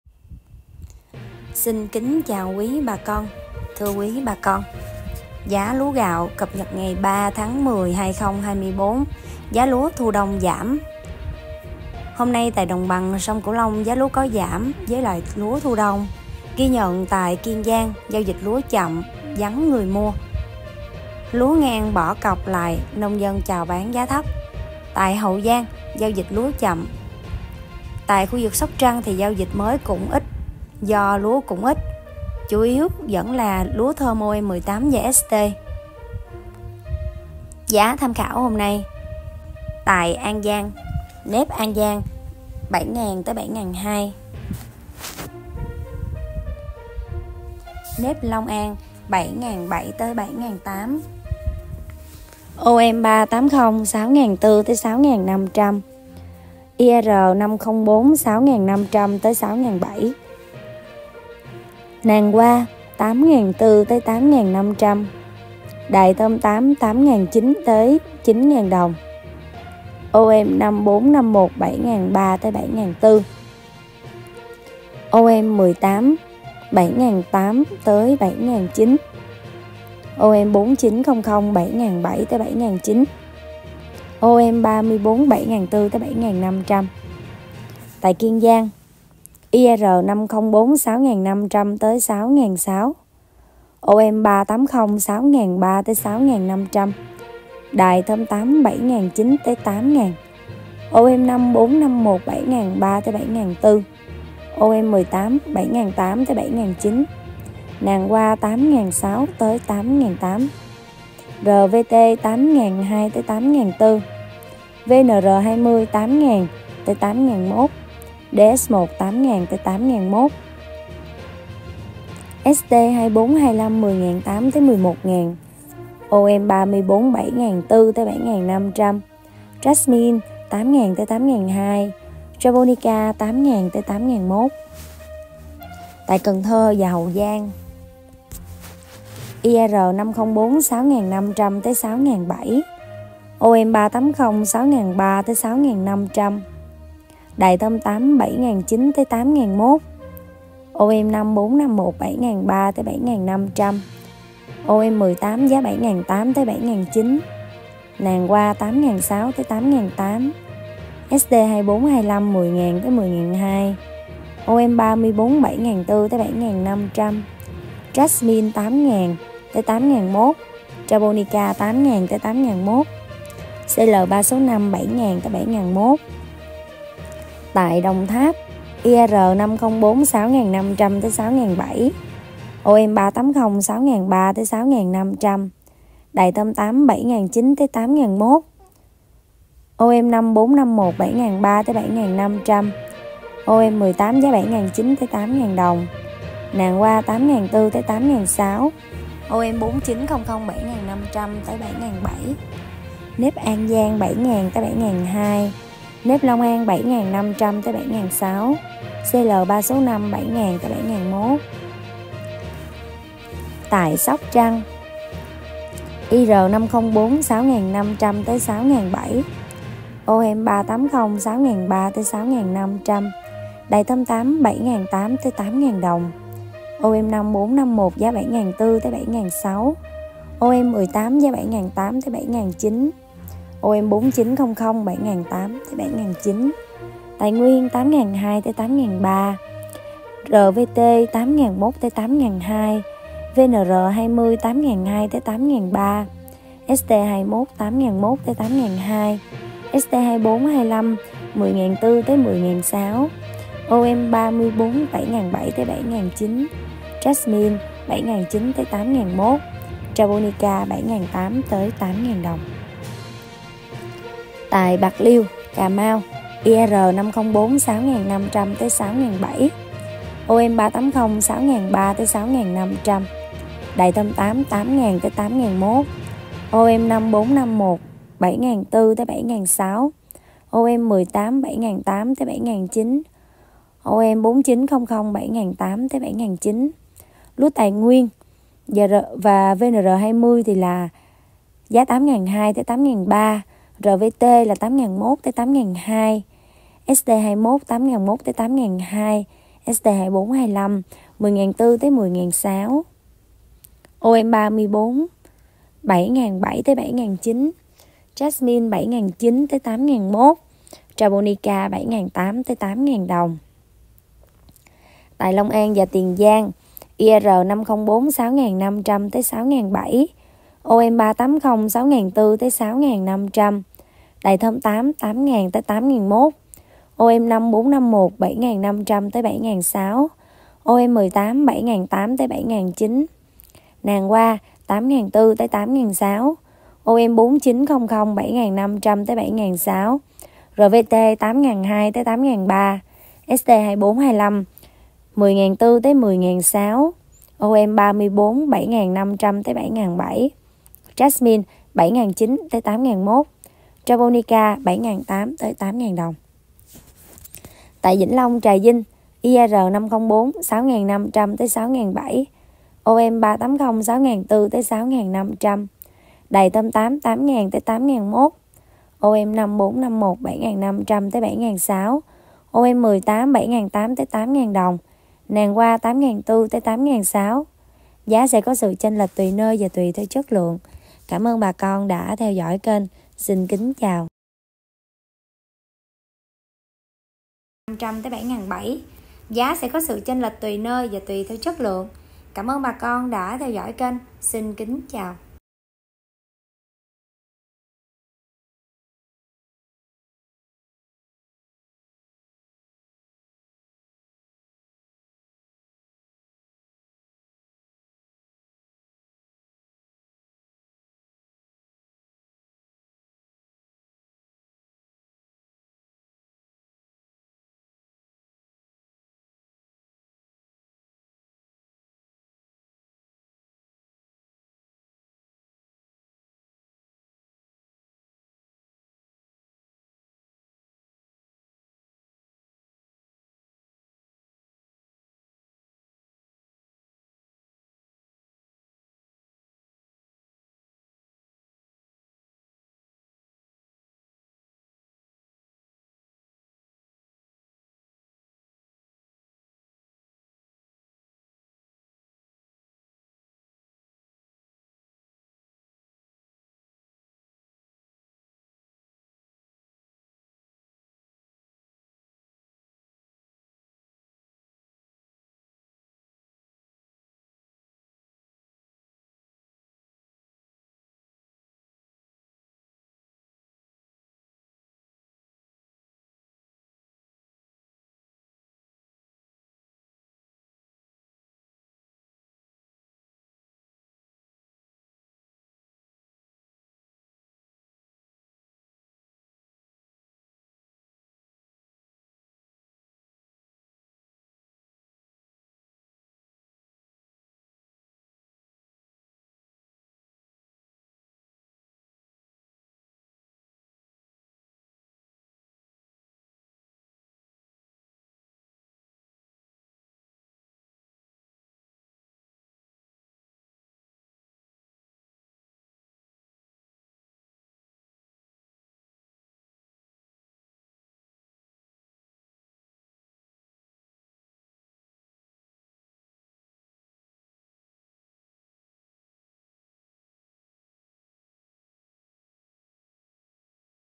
Giá lúa gạo hôm nay ngày 03.10.2024: giá lúa thu đông giảm
Giá lúa hôm nay tại ĐBSCL giảm với lúa thu đông. Ghi nhận tại Tại Kiên Giang, giao dịch lúa vẫn chậm, vắng người mua, lúa ngang bỏ cọc lại nông dân chào bán giá thấp.  Tại Hậu Giang, giao dịch lúa chậm, ít người mua. Giao dịch lúa mới tại Sóc Trăng có ít do lúa còn ít, chủ yếu lúa thơm OM18 và ST.  Giá lúa Japonica tại Hòn Đất (Kiên Giang) tiếp tục giữ ổn định, giao dịch chậm